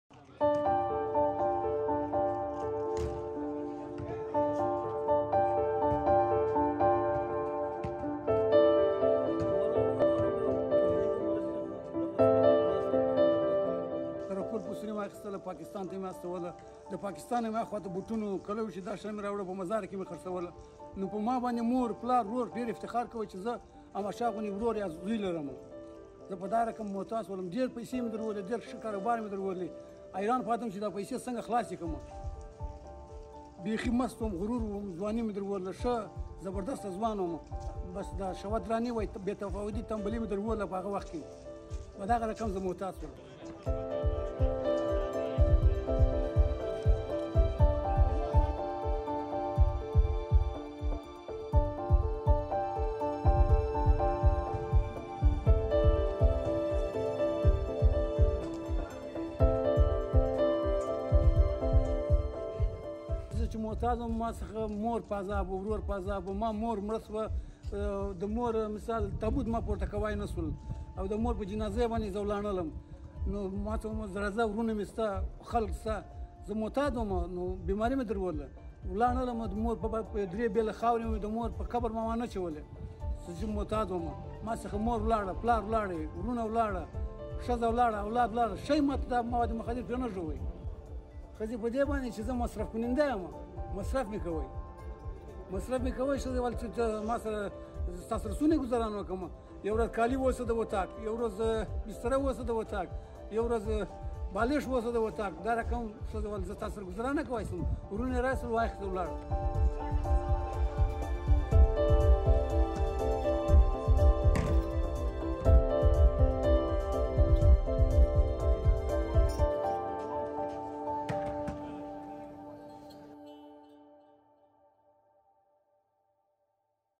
اعتیاد له خپلې کورنۍ او بچیانو جلا کړم – یو معتاد
د مزارشریف په ښار کې د معتادینو د درملنې په یو کمپ کې تر درملنې لاندې یو معتاد چې مخکې یې تجارت کاوه وايي په نشه یي توکو له روږدي کېدو وروسته یې نه یوازې خپله ټوله سرمایه له لاسه ورکړه بلکې له خپلې کورنۍ او میرمنې هم جلا شوی دی.